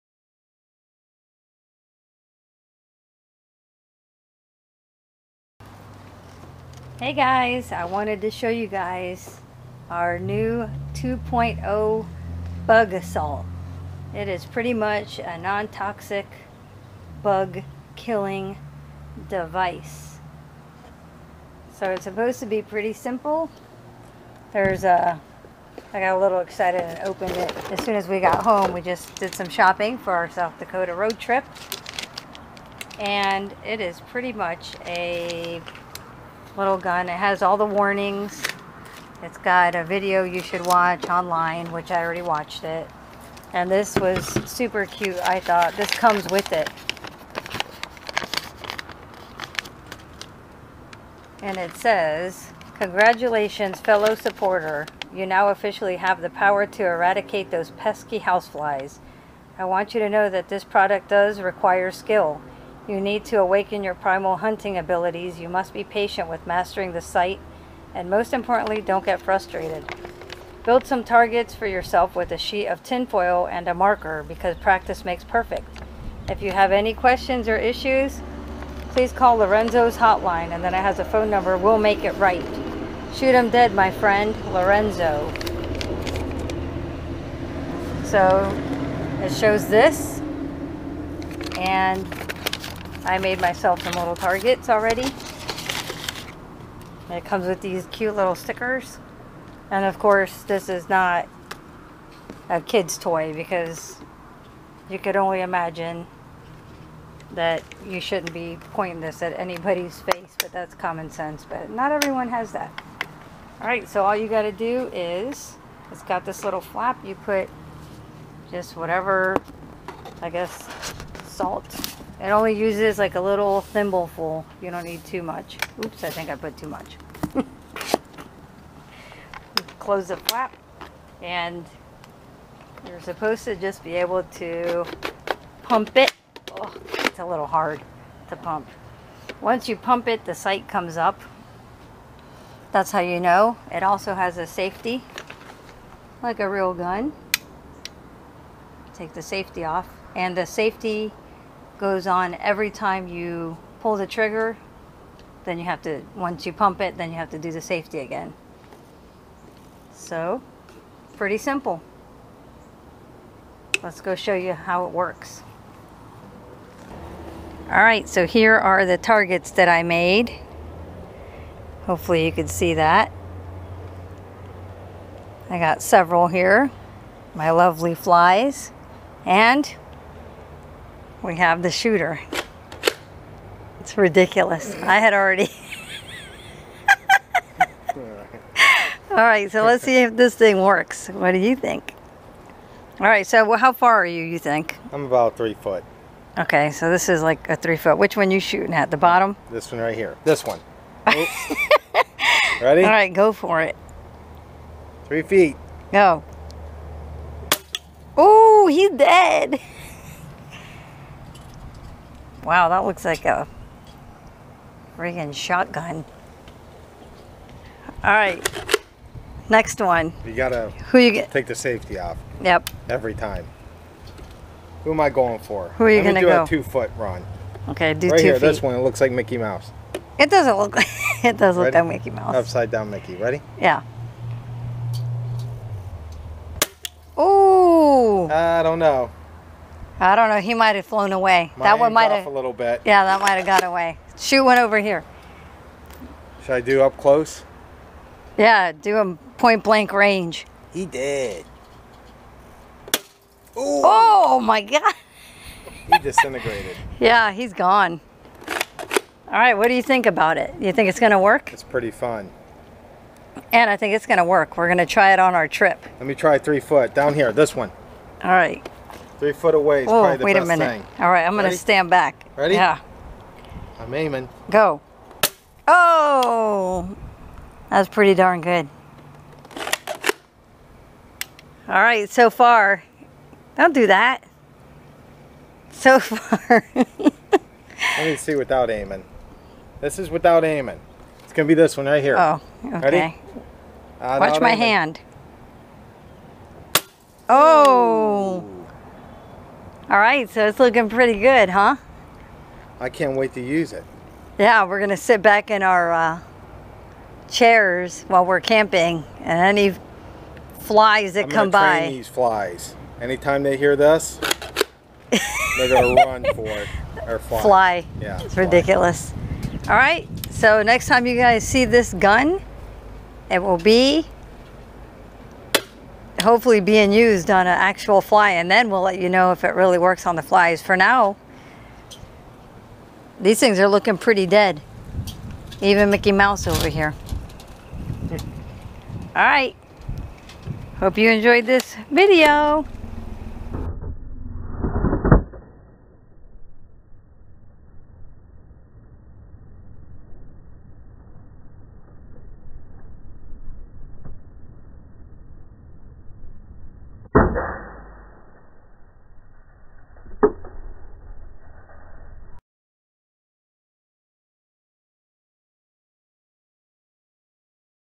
BUG-A-SALT 2.0 review !!!!! :D
We wanted to share our new "toy" the Bug-A-Salt 2.0 after looking up reviews we decided to buy it on amazon for about $50.00. This gun also claims that it won't splatter the house flies so there won't be the mess of a fly swatter :) You can also get angles that would be difficult to get with a fly swatter ( corners ). We hope to use this on our next roadtrip which will start tomorrow :) We can't wait to see if it really works.